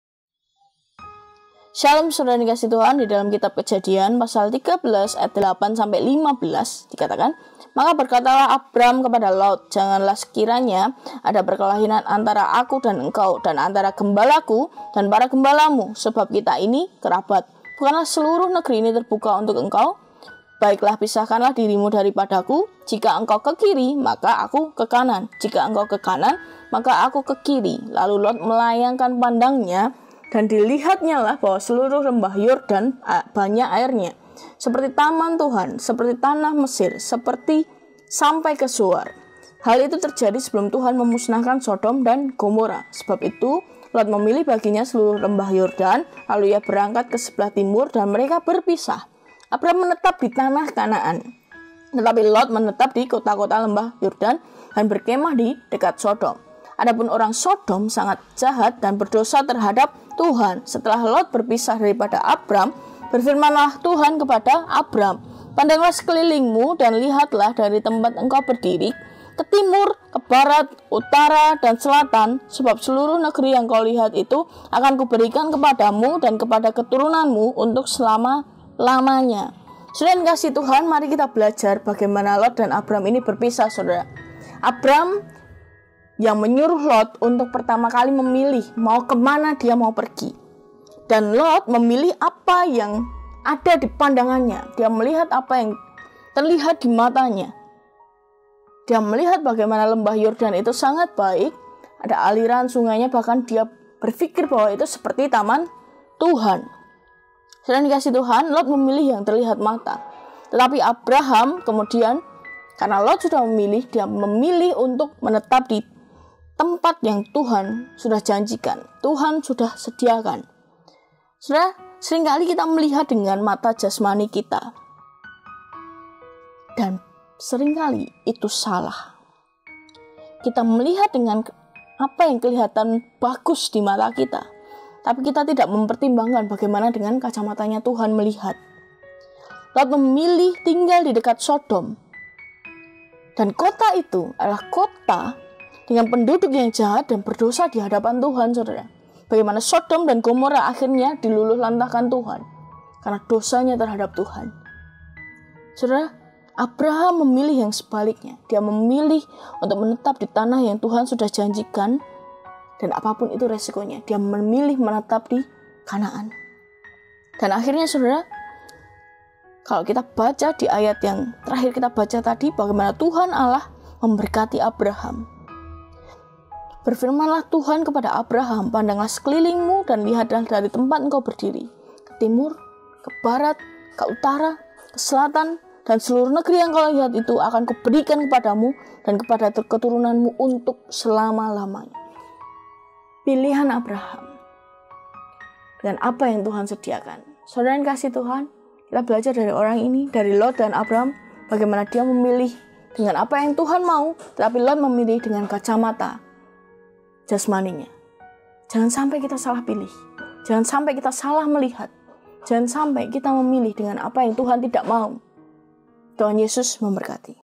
Jangan Salah Pilih | Renungan Saat Teduh 22 November 2021
Renungan dalam saat teduh ini merupakan renungan yang berisi Kesaksian, perenungan pribadi tentang Tuhan, dan tentu pengalaman-pengalaman bersama dengan Nya.
 Ditulis oleh Dwi Nurhayati